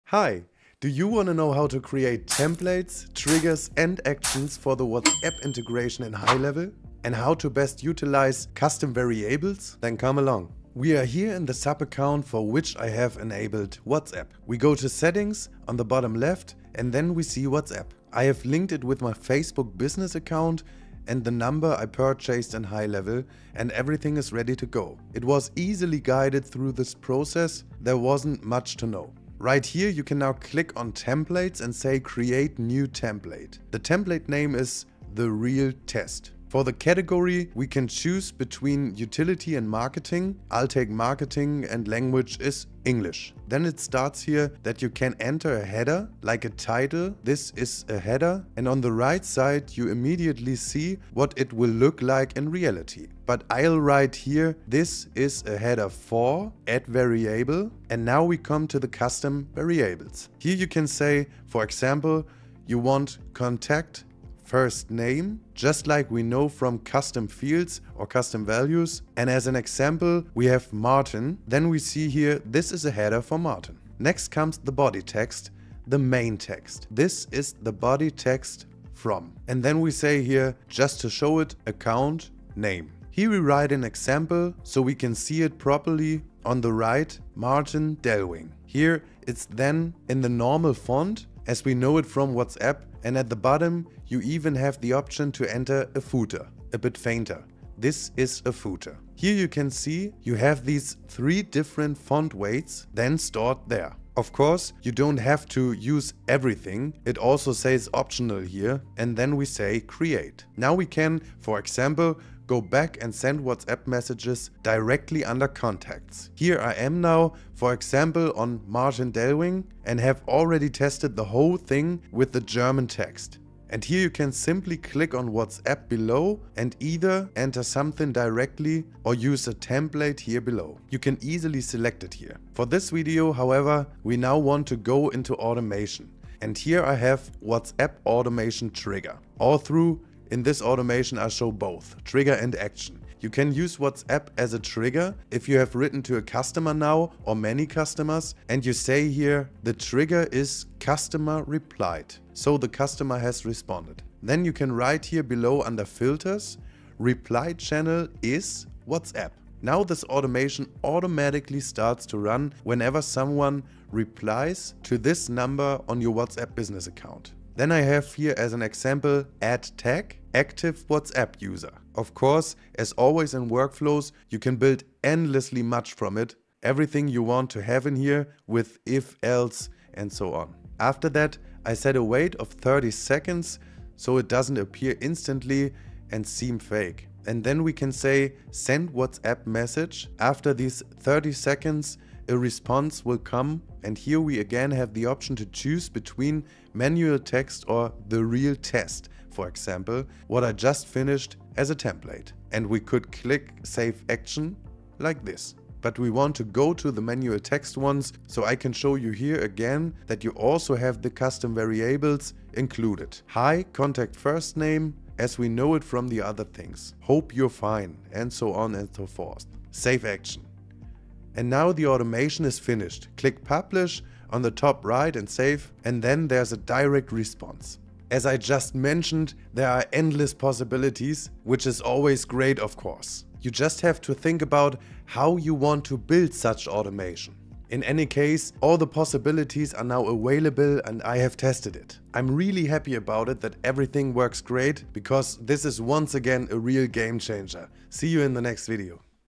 WhatsApp Templates, Triggers, Actions & Custom Variables in HighLevel
In this video, I'll show you how you can use custom variables for your WhatsApp marketing in HighLevel. We also go into templates and the workflow options of WhatsApp in HighLevel.
🎁 30 DAYS FREE HIGHLEVEL UNLIMITED OR PRO TEST

🚀 YOU ALREADY HAVE AN ACCOUNT BUT WANT TO JOIN MY TEAM?